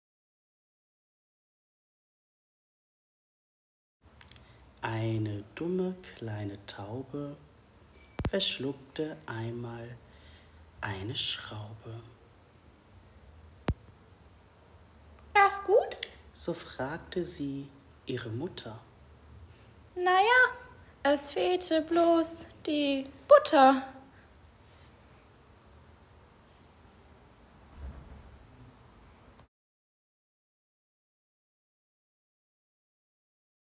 Taube
Lehramts-Studierende der Uni Siegen haben bei einer Exkursion nach Bremen einen Workshop bei uns besucht. Aus Gedichten und Kunstwerken entstanden am iPad Trickfilme.